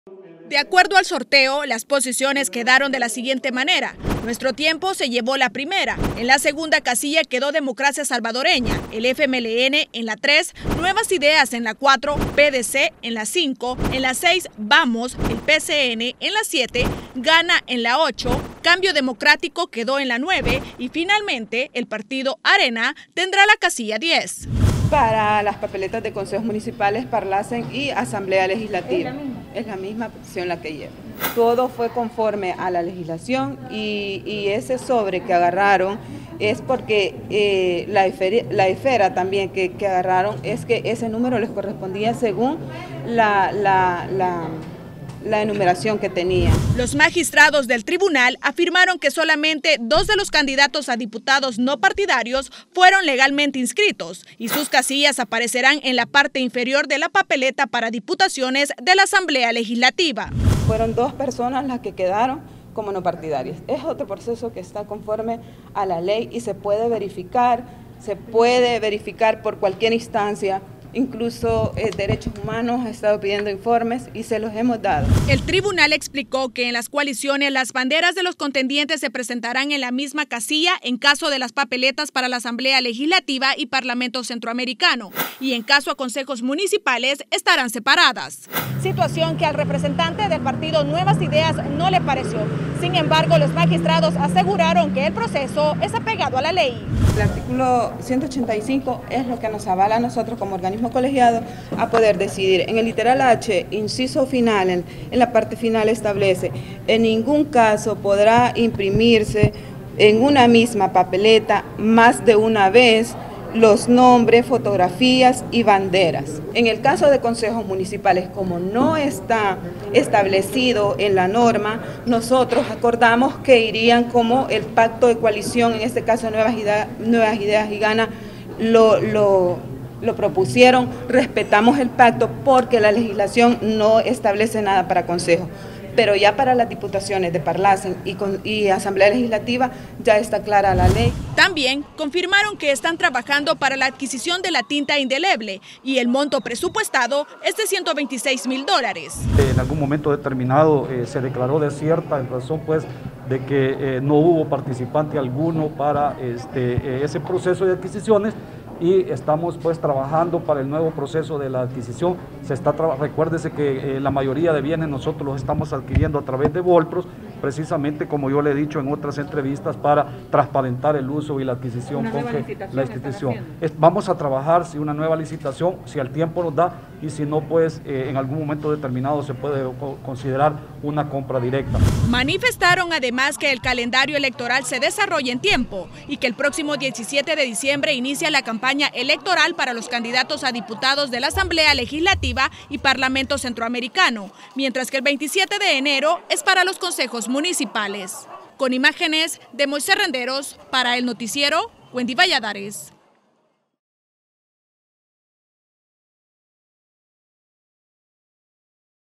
TSE sorteo posición de banderas en papeletas de votación
El Tribunal Supremo Electoral, sorteo la posición de las banderas de los partidos políticos para las tres papeletas de votación del 2021, quedando en la primera posición el partido nuestro tiempo, en la siguiente nota los detalles.